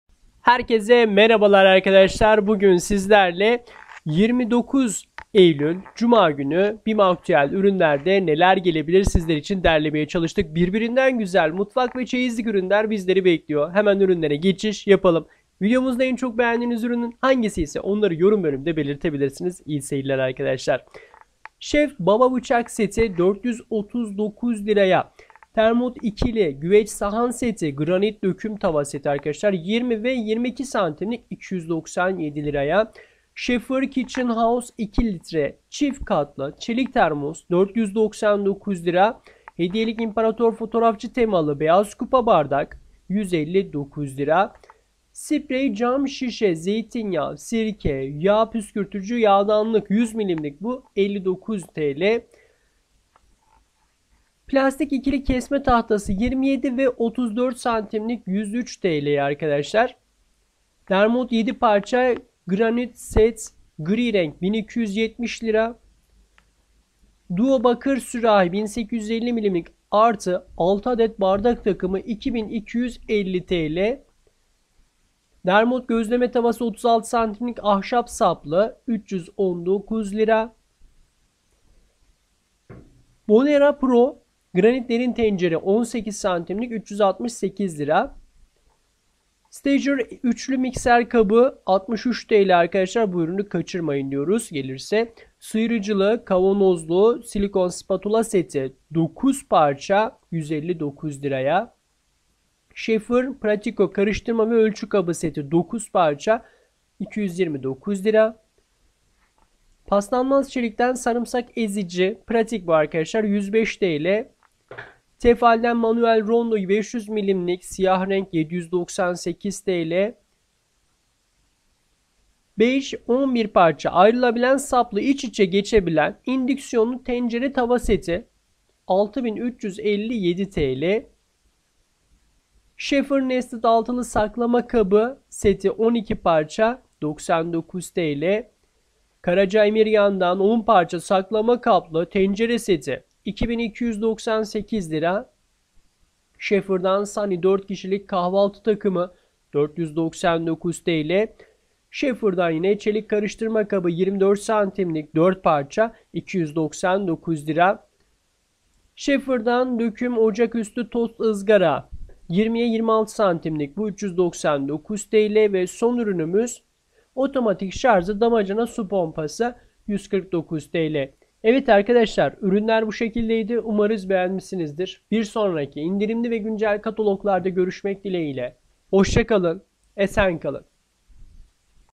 Bim Bu Hafta Cuma Neler Var - 29 Eylül 2023 Aktüel Ürünler Beklenen Kataloğu
Bim bu hafta cuma neler var?
Bim aktüel ürünleri bu hafta cuma günü neler var diye merak edenler İNDİRİM AKTÜEL kanalımızda buluştu.